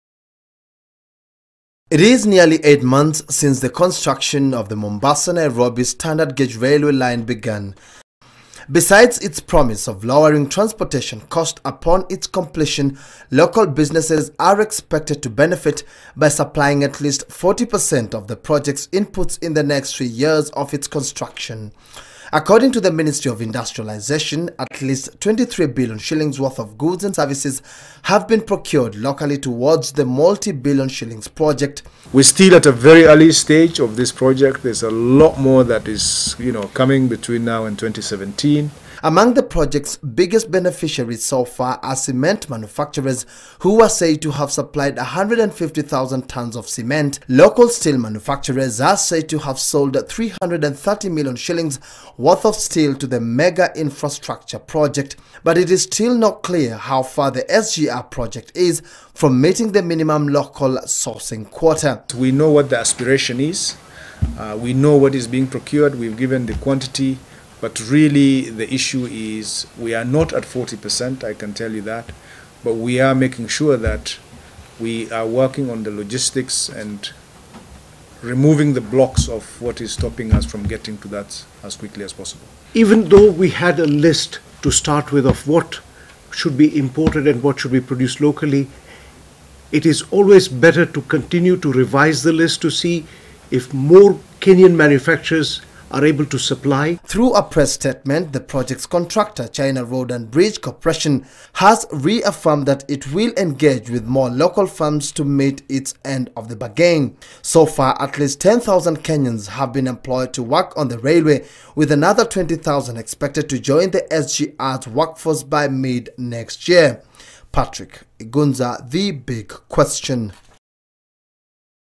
materials used in the Kenya Standard Gauge railway Project
More than 60 per cent of materials used in the ongoing Standard Gauge railway Project are imported despite the ‘buy Kenya build Kenya’ crusades.

Industrialisation Cabinet Secretary Adan Mohamed Tuesday admitted that the project heavily relies on imported materials sighting failure by local firms to produce the set standards required in the multibillion project.
“ I can confirm that we have not met the minimum 40 per cent threshold. We expect this to come out gradually as we proceed with the project ,” Mr Mohamed said Tuesday after meeting stakeholders involved in the SGR project.
The stakeholders had allegedly agreed on a list containing what should be imported and what should be sourced locally before the project kicked off.
Kenya Association of Manufacturers Chairman Pradeep Paunrana said the list will now need to be revised in light of the new developments in the local manufacturing sector.
“We had this list before and most companies may have lacked the capacity to provide the quality materials required but we have since advanced our production technology and capacity to meet the requirements. We believe this list should be audited to reflect the current reality,” Mr Paunrana said.
RAISED CONCERNS
The CS had been held in a long closed door meeting where players raised concern over the allocation of tenders to supply the materials in the Sh327 billion project.
Materials worth over Sh23 billion have been supplied by the 300 suppliers in the project said to be about 36 per cent underway. Local sourcing is said to have accounted for about sh10 billion.
According to the financial agreement signed by Kenya and China in May 2014, the East African nation is required to finance 10 per cent of the funds required to construct the 609 kilometre railway line from Mombasa to Nairobi, while China will finance 85 per cent of the total cost through the Export and Import (Exim) Bank of China.
President Uhuru Kenyatta in his 2015 Madaraka Day speech announced new procurement changes that would require all government purchases to have 40 per cent local goods.
“My government shall in the new financial year strictly ensure that as a minimum, 40 percent of all goods and services procured by government at all levels are locally produced. Additional policies to open up new sectors such as the creative arts and sports sectors are being earnestly pursued,” Mr Kenyatta said.
The 2015/2016 budget proposals had advanced similar measures. Treasury Cabinet Secretary Henry Rotich put on notice public entities that are yet to comply with the requirements of Build-Kenya, Buy-Kenya to do so with immediate effect.
“The requirement covers all procurement in respect of road works, railway works, airports and other ports and harbours, works and materials for generation, transmission and distribution of energy and other construction materials made in Kenya," Mr Rotich said when he presented the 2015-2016 budget estimates